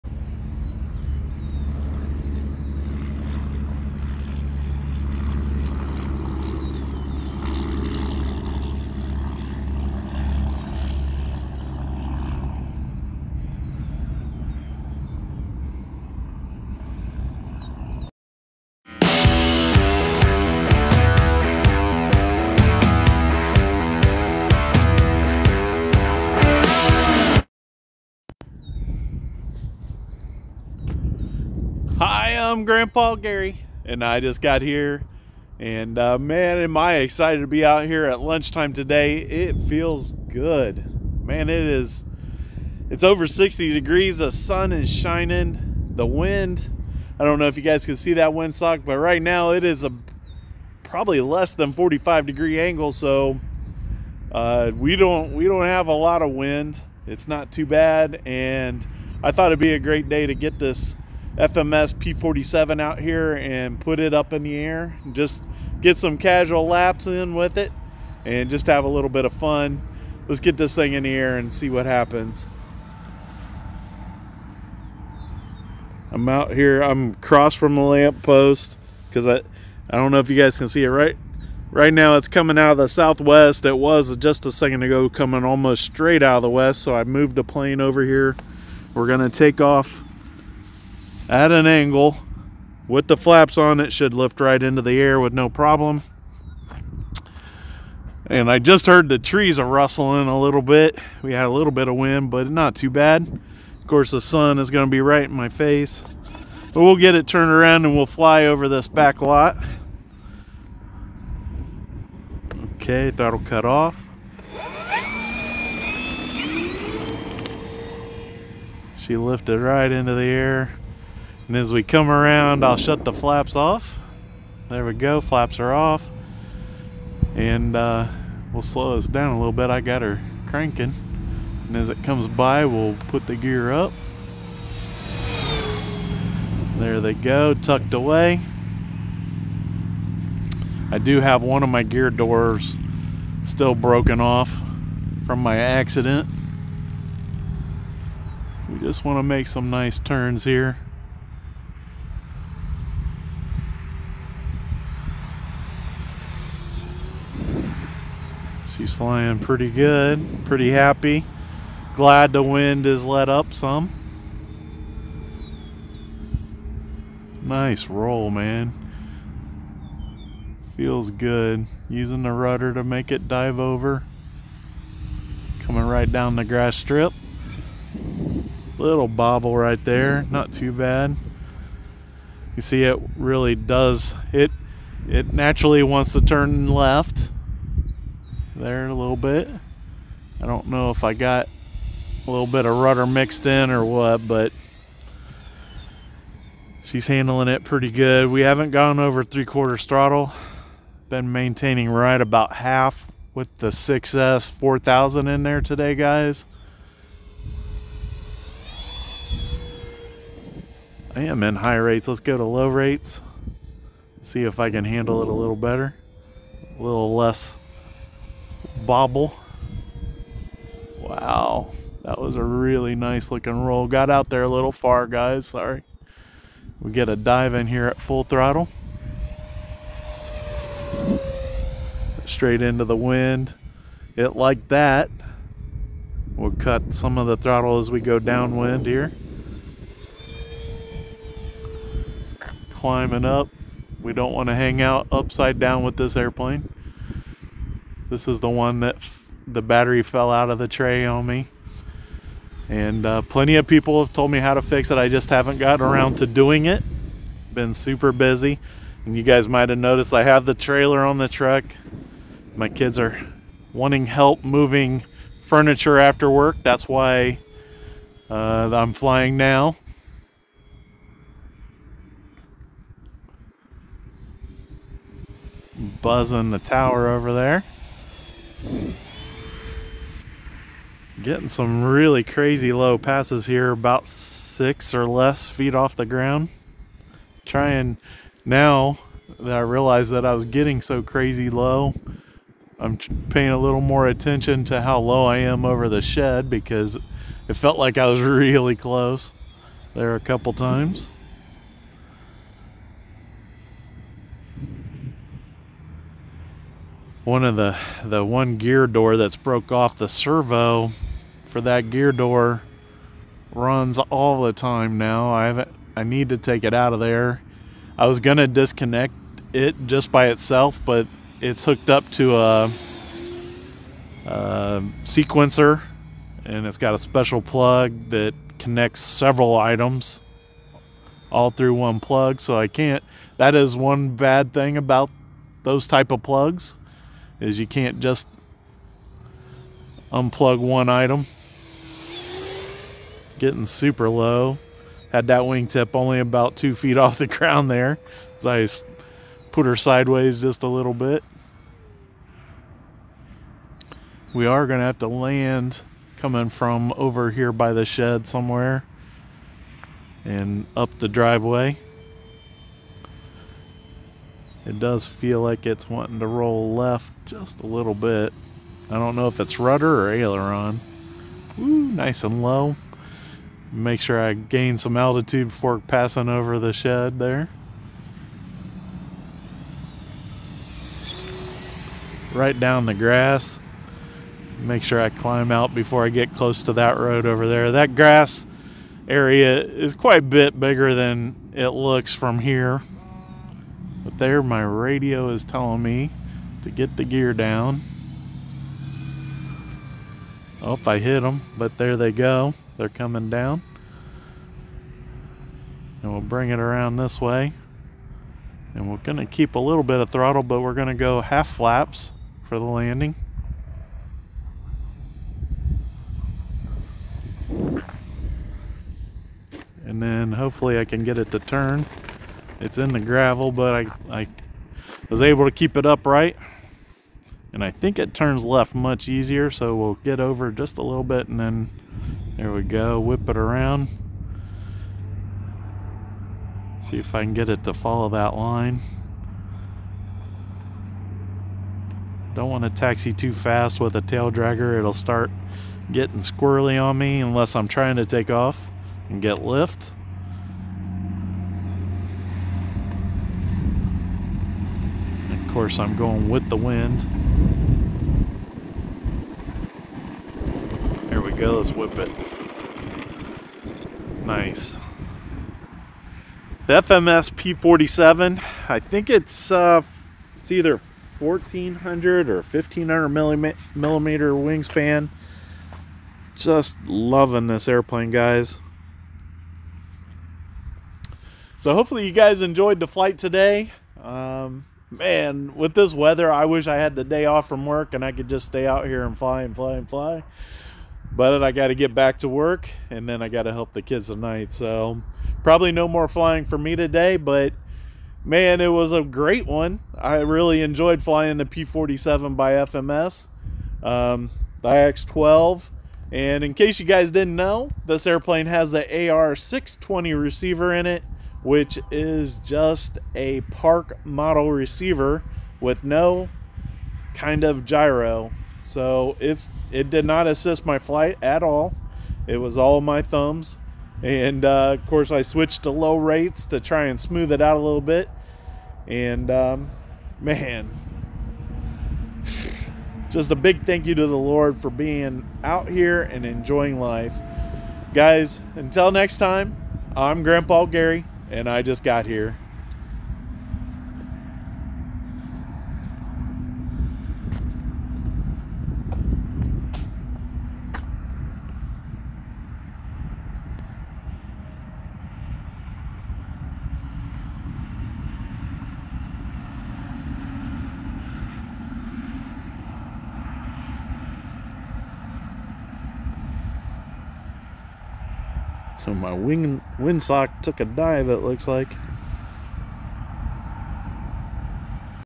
Casual Flight of the FMS P-47 Razorback! (Live)
On February 4th 2020, I completed 365 days straight of flying RC planes outside.  Today I continue a repeat of flying 365 days straight.  This is a live video of me flying the FMS P-47 Bonnie.

David Chenzoff
Kevin Trappen
Anthony May
Wayne Grimstead
Dustin Kremer
Michael Jones
Eugene Casiere